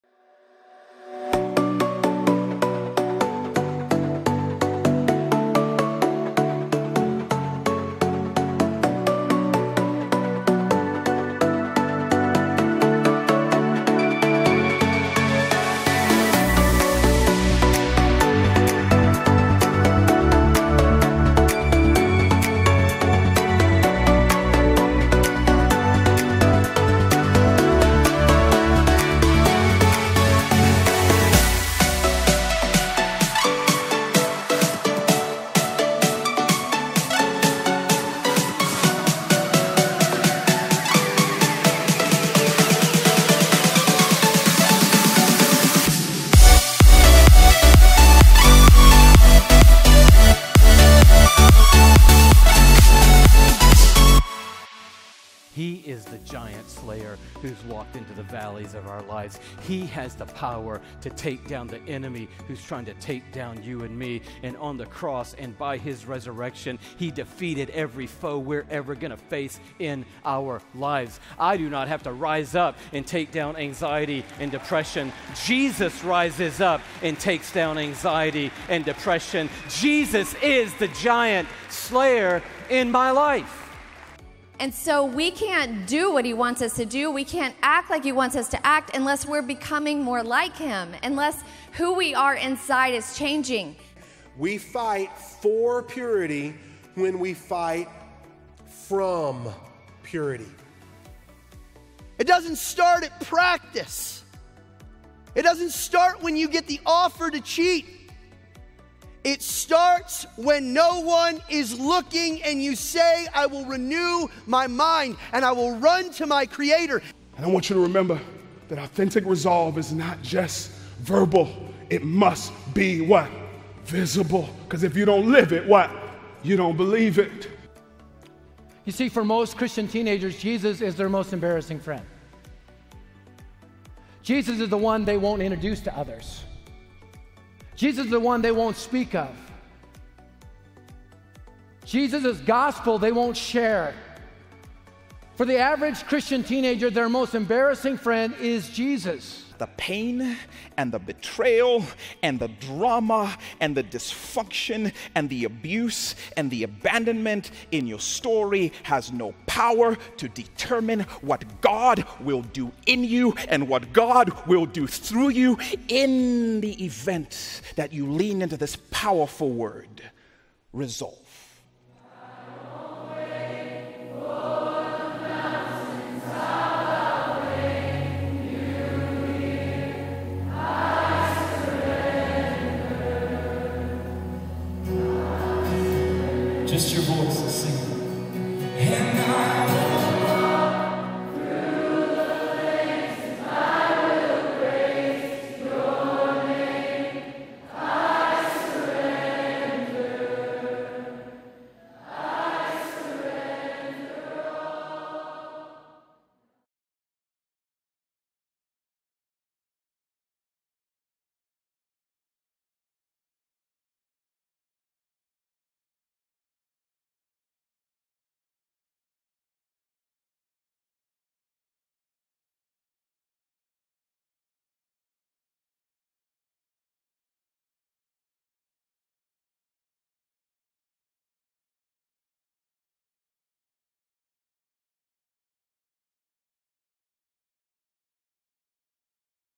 Momentum Youth Conference 2018 Recap Video
Momentum, a youth conference for students in 6th-12th grade, is produced by CE National. At CE National we believe very strongly in youth and truly desire to see them develop a heart for God and for His Word. Find out more about Momentum at buildmomentum.org.